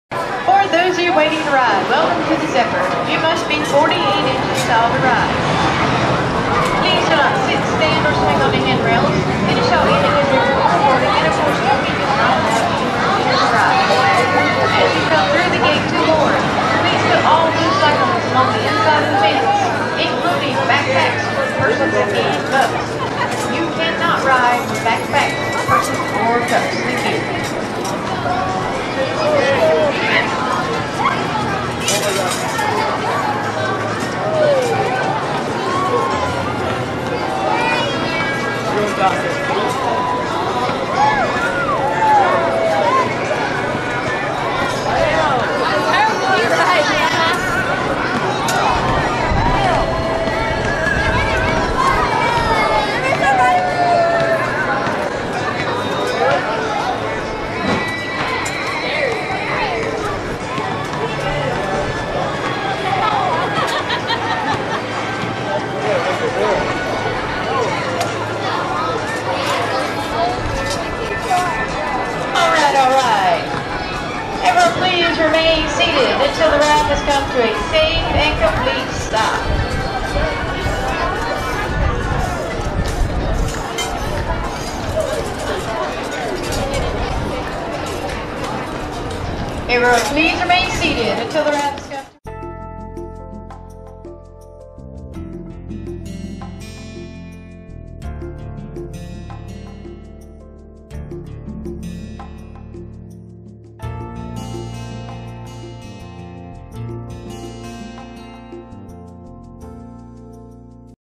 Zephyr at Carowinds 2017
Zephyr at Carowinds 2017. Annalisa and her mom ride the new Zephyr ride at Carowinds theme park in Charlotte, North Carolina!